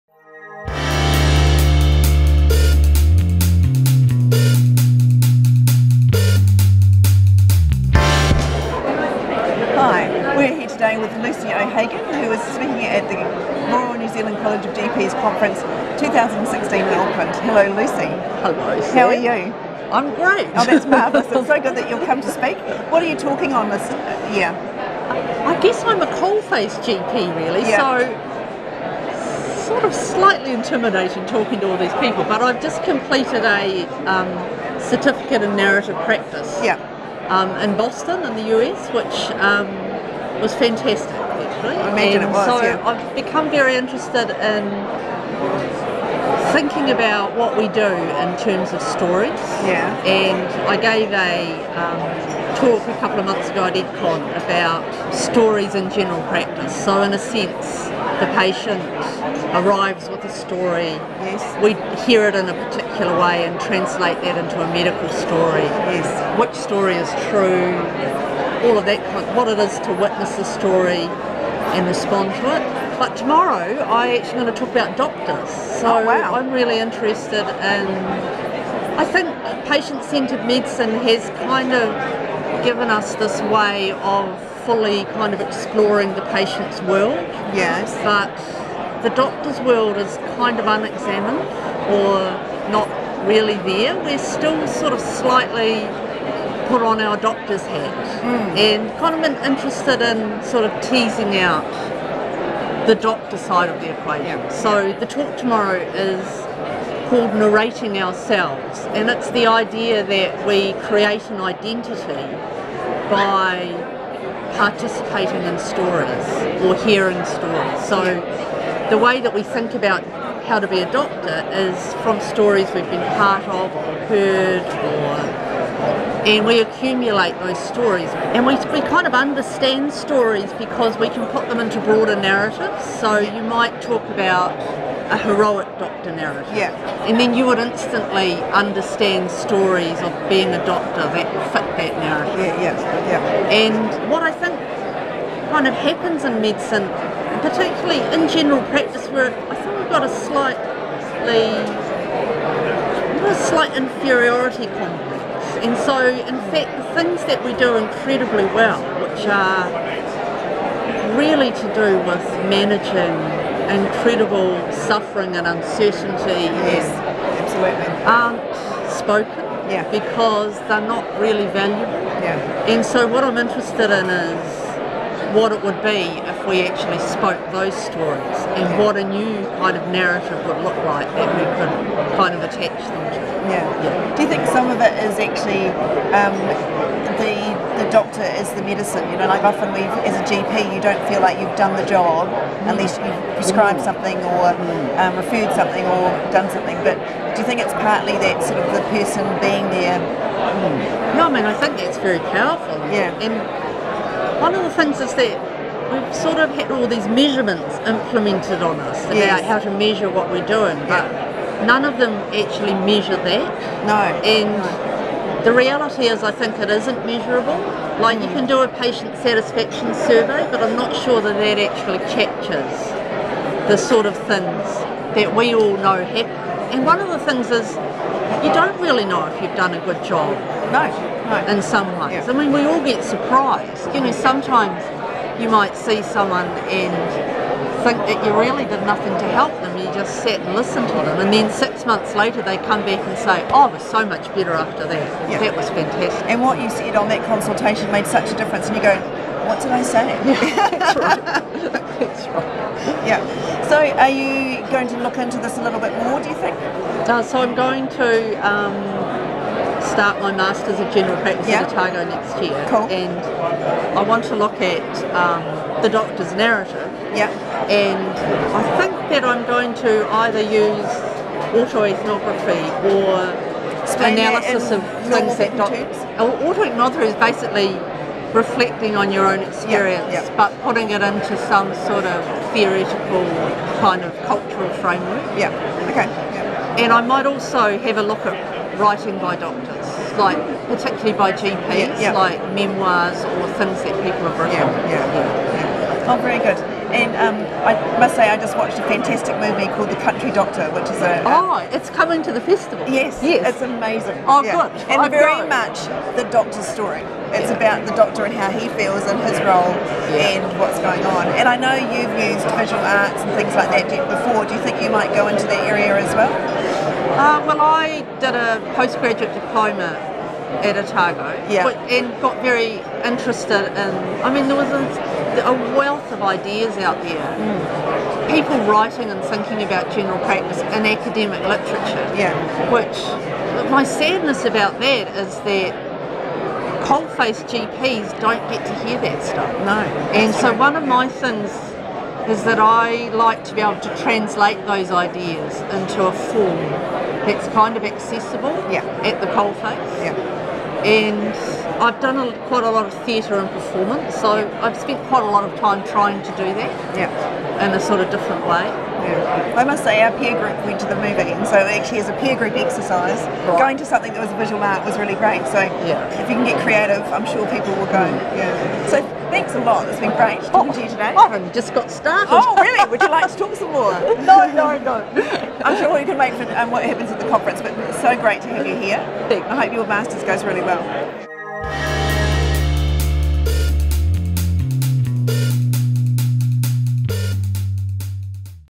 Lucy OHagen - Conference for General Practice
Lucy Discusses the Certificate in Narrative Practice that she has just completed.